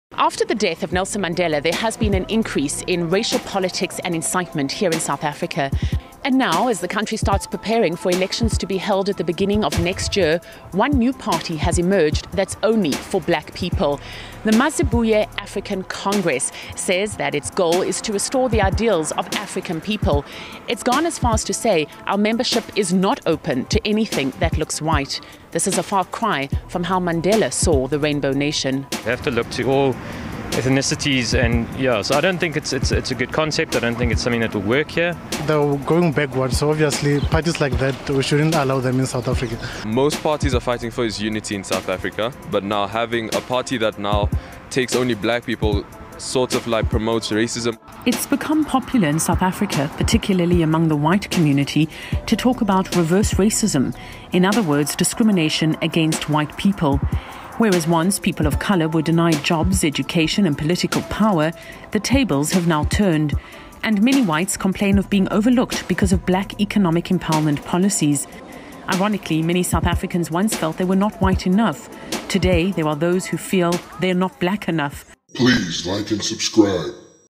Mazibuye African Congress Of South Africa
In the wake of government plans to change the constitution and strip white farmers of their land, some South African politicians are looking for more black emancipation. A new party has made headlines after banning whites and foreigners from becoming members.